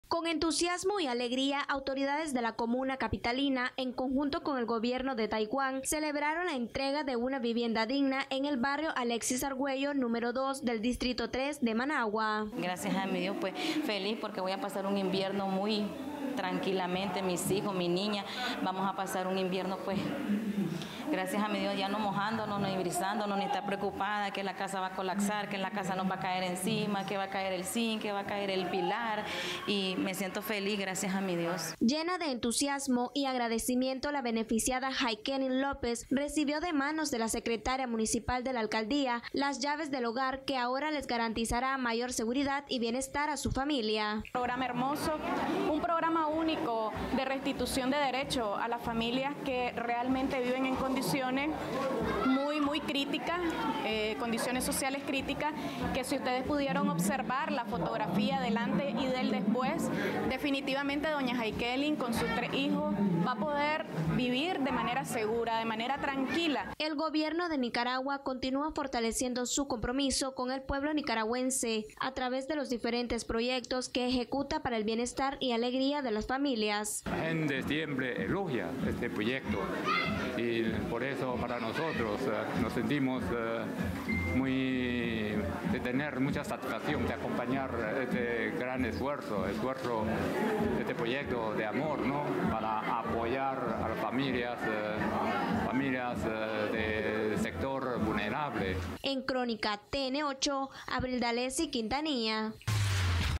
Comuna capitalina llena de felicidad a familia del barrio Alexis Argüello - Nicaragua
Con entusiasmo y alegría autoridades de la comuna capitalina en conjunto con el gobierno de Taiwán, celebraron la tarde de este martes la entrega de una vivienda digna en el barrio Alexis Argüello N°2, del Distrito III de Managua.

"Yo me siento muy feliz porque yo antes vivía en una casa que casi se caían los pilares, que se me pasaba el agua y yo vivía pensando en que mis niñas se iban a mojar, en que en algún momento en la casa se iba a caer y durante el huracán fue ahí donde llegó el tope y por las fuertes lluvias e inundaciones la casa se me derrumbó totalmente", expresó Hayquenin López, beneficiada.

"Hoy gracias a mi Dios estoy muy feliz porque voy a pasar un invierno muy tranquilo y mis hijos y mi niña van a pasar un invierno ya no mojándonos, ni brisando, ni estando preocupada de que la casa va a colapsar o que nos va a caer encima", agregó la beneficiada.

Llena de entusiasmo y agradecimiento la beneficiada Hayquenin López recibió de manos de la secretaría municipal de la Alcaldía de Managua las llaves del hogar, que ahora les garantizará mayor seguridad y bienestar a su familia.

Una nueva vida
"Este un programa hermoso, un programa único de restitución de derechos a las familia que realmente viven en condiciones muy críticas, en condiciones sociales críticas que si se pudiera observar la fotografía de antes y después, definitivamente con su hijo va a poder vivir de manera segura, de manera tranquila", compartió Jennifer Porras, de la secretarií municipal de la Alcaldía de Managua.

El Gobierno de Nicaragua continúa fortaleciendo su compromiso con el pueblo nicaragüense a través de los diferentes proyectos que ejecuta para el bienestar y alegría de las familias.

"Este proyecto nos hace sentir que tenemos mucha satisfacción porque podemos acompañar este gran esfuerzo y de este proyecto de amor para apoyar a las familias que son del sector más vulnerable, y que sufren a diario porque no pueden reconstruir o tener sus casitas en buen estado", finalizó el Emabajdor de Taiwán en Nicaragua.